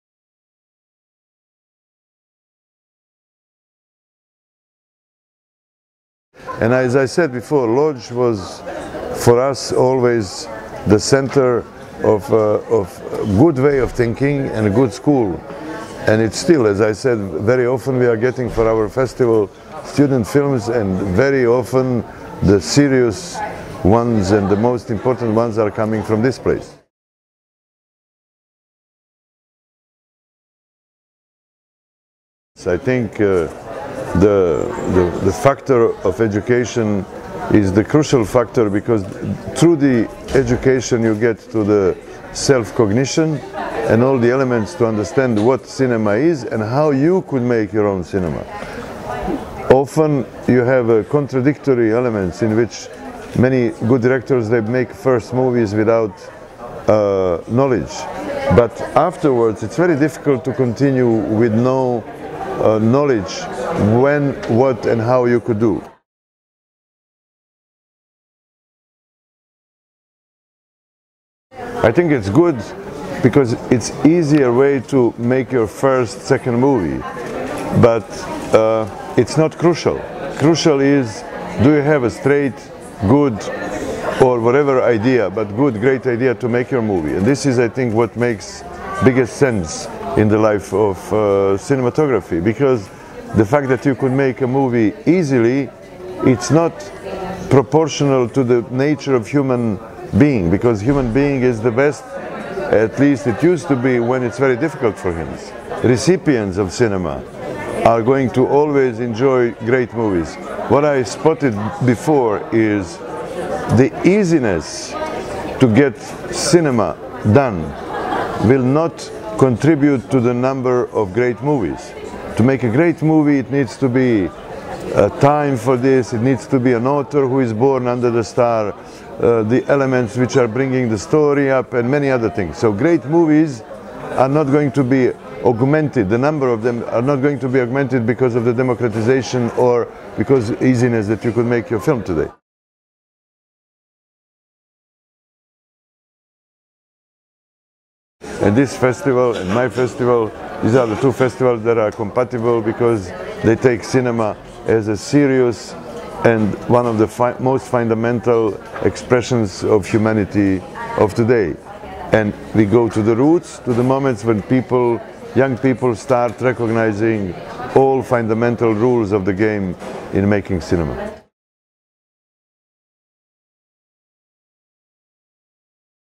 EMIR KUSTURICA - TARGOWA STREET FILM&MUSIC FESTIVAL 2011
0:04 O "filmówce"
0:29 Edukacja filmowa a tworzenie filmów
1:10 Dostępność sprzętu a jakość filmów
2:24 Przyjaźń festiwali Kustendorf i Targowa

Najwybitniejszy serbski reżyser filmowy, scenarzysta, epizodycznie aktor, muzyk. Dwukrotnie nagrodzony Złotą Palmą na Festiwalu Filmowym w Cannes (1985, 1995). Przyjaciel Targowej od pierwszej edycji, honorowy patron konkursu etiud filmowych.

Na festiwalu TARGOWA STREET FILM&MUSIC FESTIVAL 2011 Gość Specjalny. Poprowadził również spotkanie z cyklu LEKCJE KINA Z MISTRZAMI.

Odbywający się w styczniu w Kustendorfie festiwal filmowy Emira -- Kustendorf Film & Music Festival -- jest od roku 2009 festiwalem partnerskim Targowej.